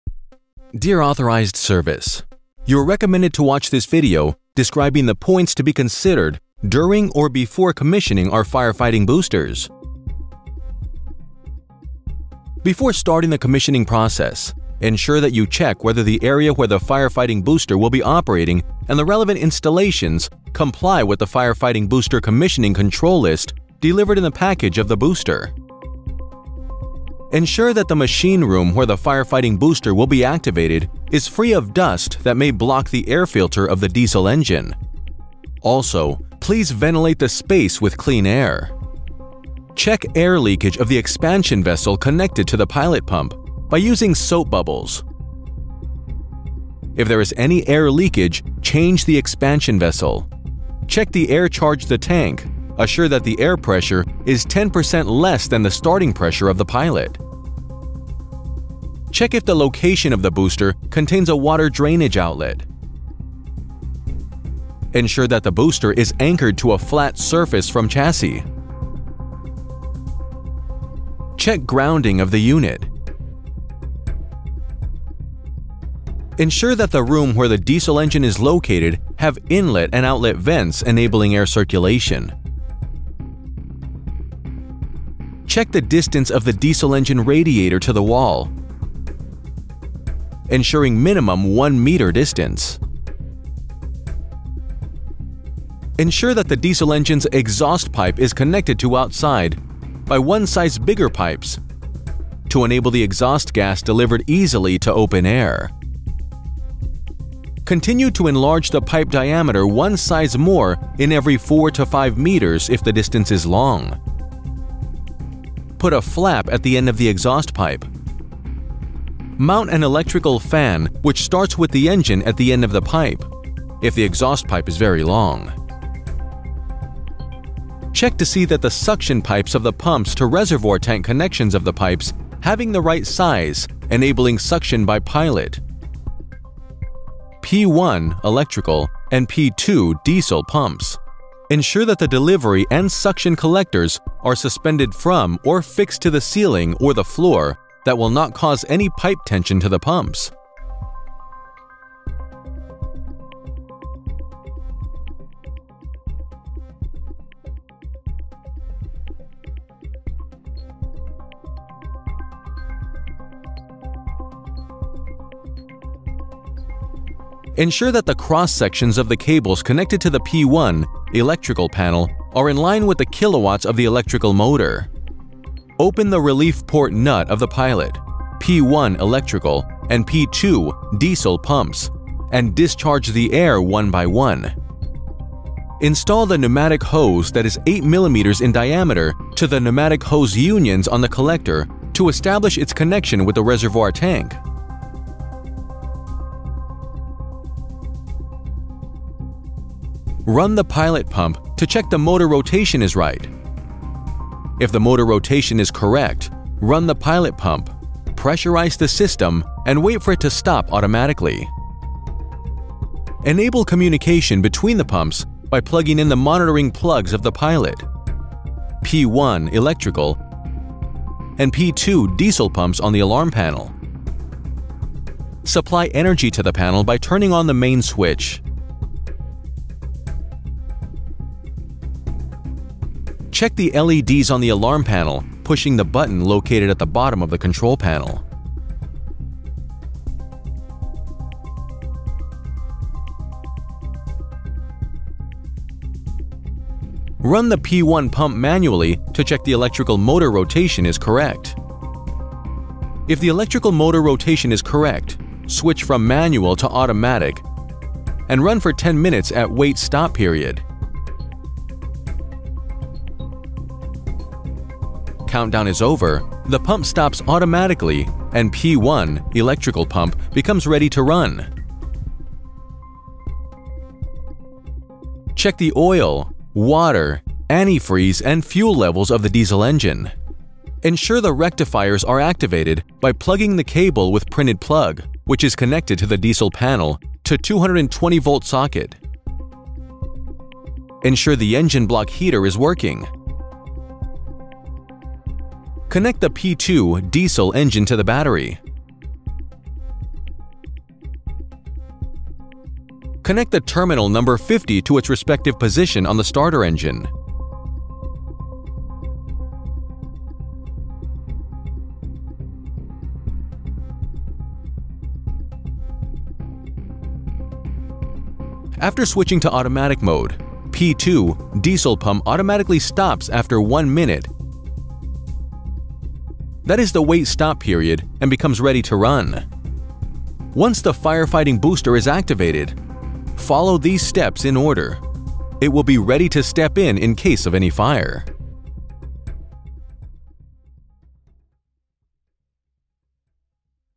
Commissioning of Fire Fighting Boosters
You can learn about the steps to be considered during the commissioning of our booster systems by watching our instructional video on "Commissioning of Fire Booster Boosters", which we have prepared for our authorized services.